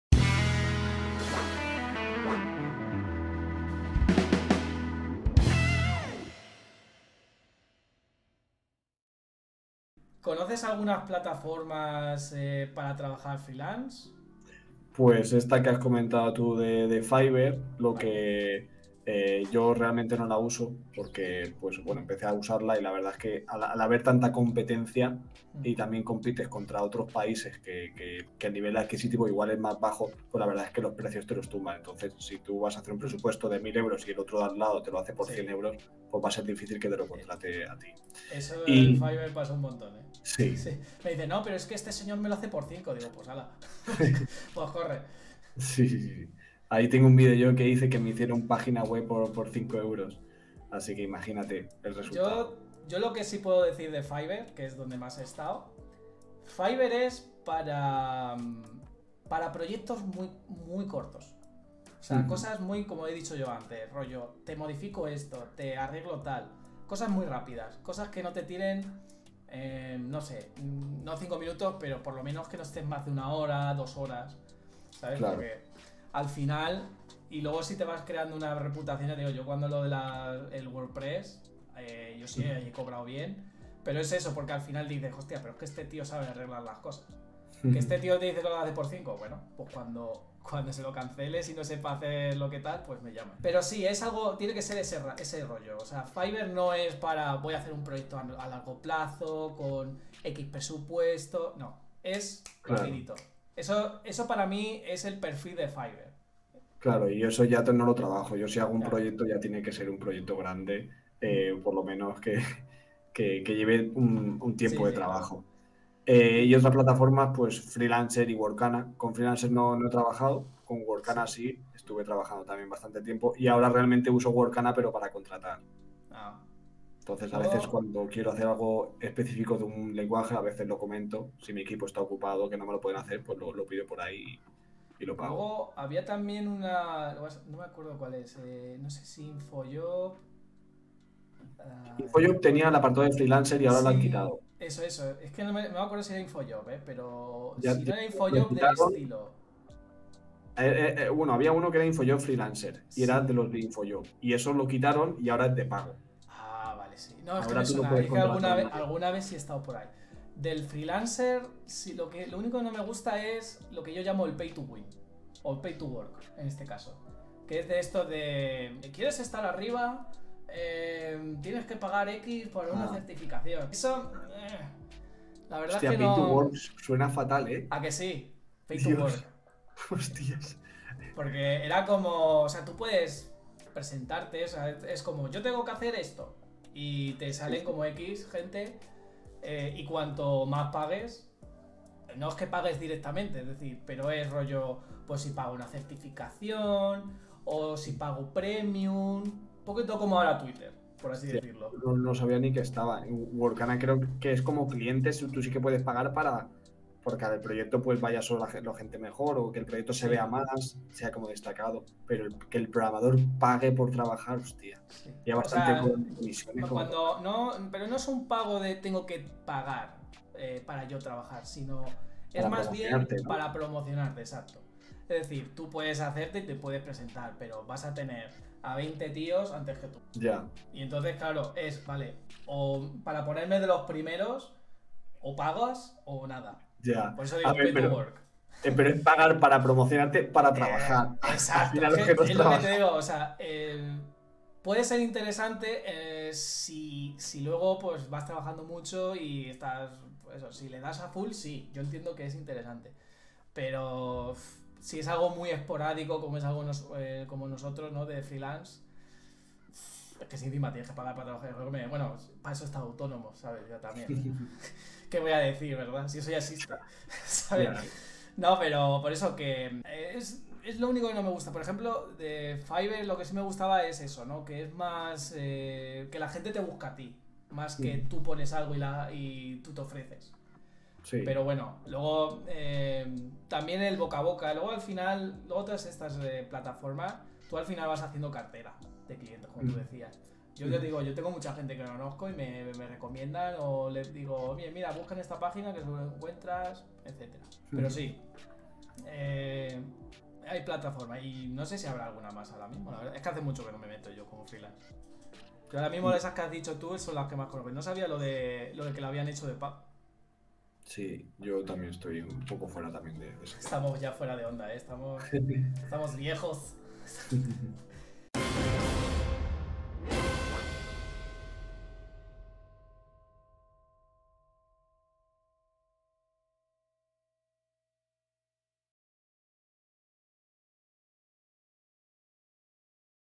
¿Qué PLATAFORMAS FREELANCE recomiendas para trabajar?
¿Quieres empezar como freelance? Randy Varela te recomiendo algunas plataformas freelance para empezar.

- Enlaces